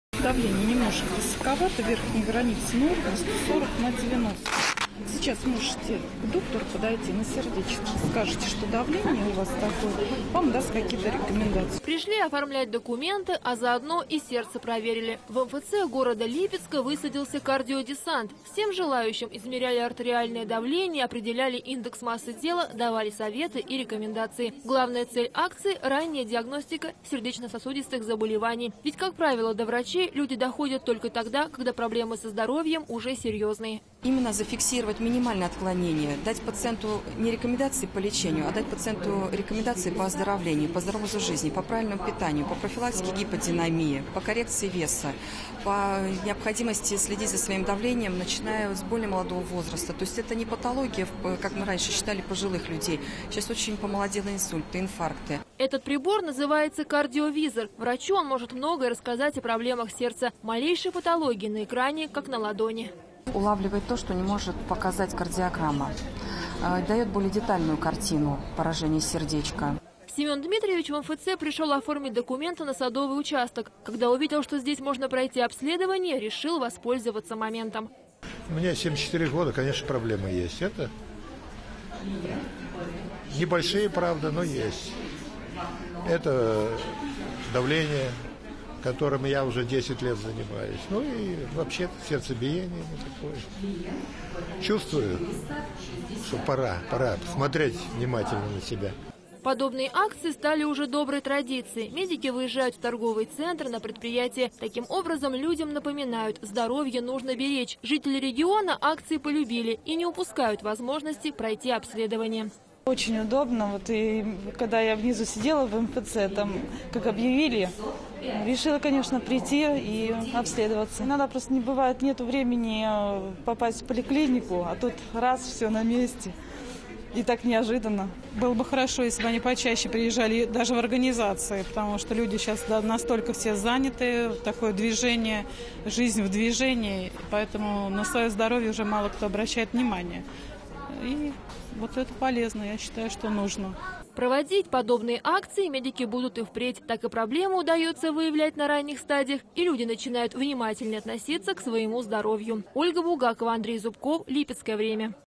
В липецком МФЦ провели акцию "Кардиодесант"...
Посетители супермаркета услуг могли пройти медобследование.